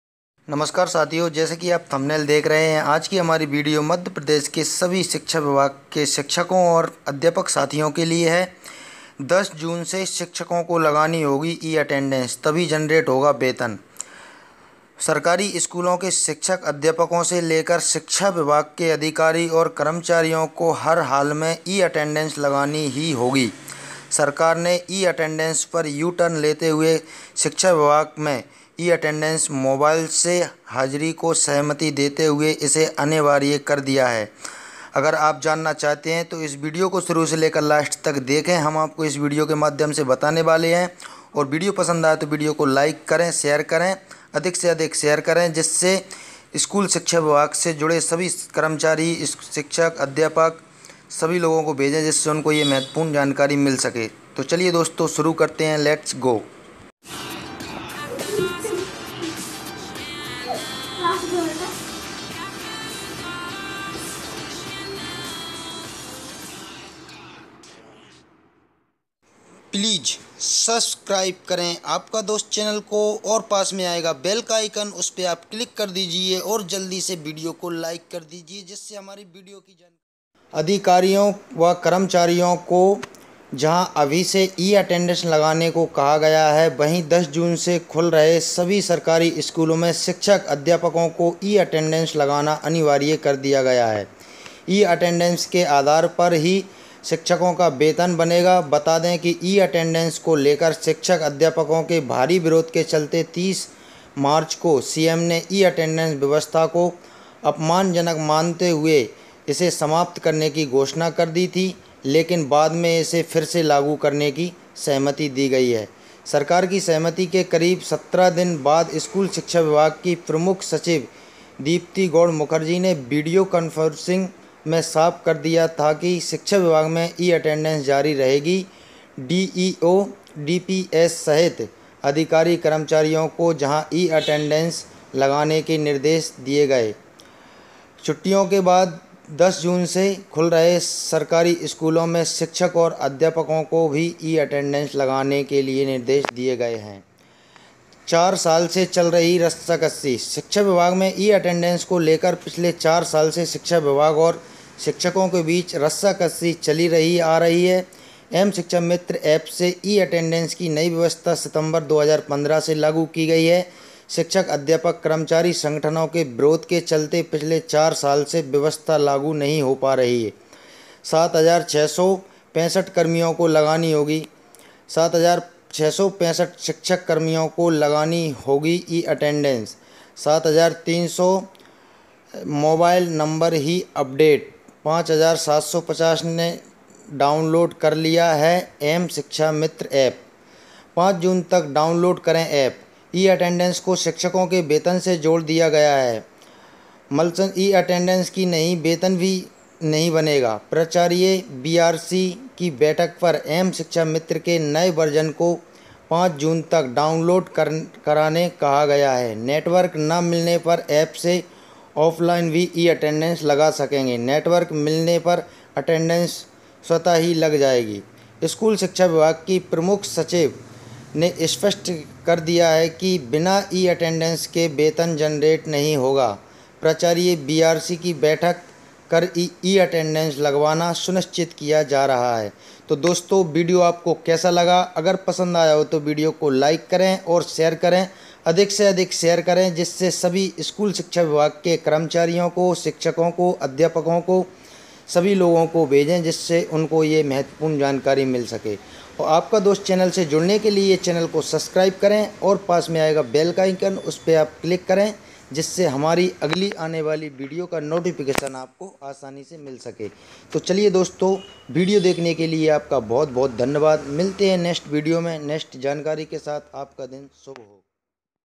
10 जून से शिक्षकों को लगानी होगी।ई-अटेंडेंस तभी जनरेट होगा वेतन।Apka Dost।Teachers will have to appl
Teachers will have to apply from June 10. E-Attendance will only generate salary. Apka Dost10 जून से शिक्षकों को लगानी होगी।ई-अटेंडेंस तभी जनरेट होगा वेतन।Apka Dost।